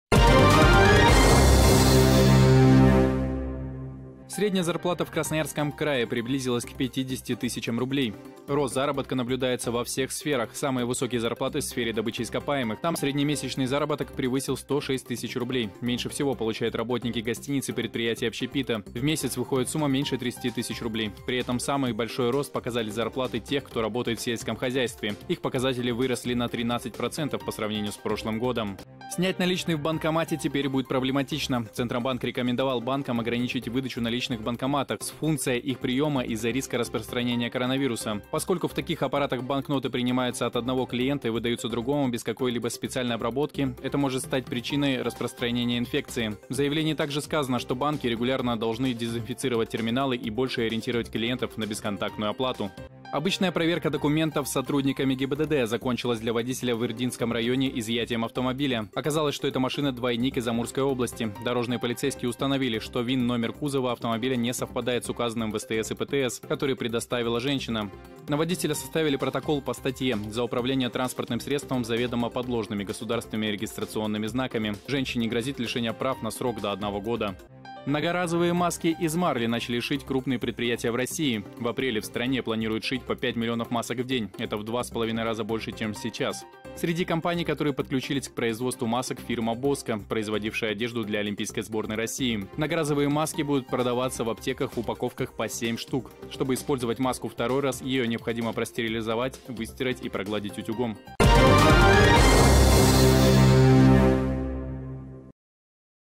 Коротко о разном 24.03.2020
Средняя зарплата в Красноярском крае приблизилась к 50 тысячам рублей

Рост заработка наблюдается во всех сферах. Самые высокие зарплаты в сфере добыче ископаемых. Там среднемесячный заработок превысил 106 тысяч рублей. Меньше всего получают работники гостиниц и предприятий общепита — в месяц выходит сумма меньше 30 тысяч рублей. При этом самый большой рост показали зарплаты тех, кто работает в сельском хозяйстве — их показатели выросли на 13% по сравнению с прошлым годом.

О других новостях дня в рубрике «Коротко о разном».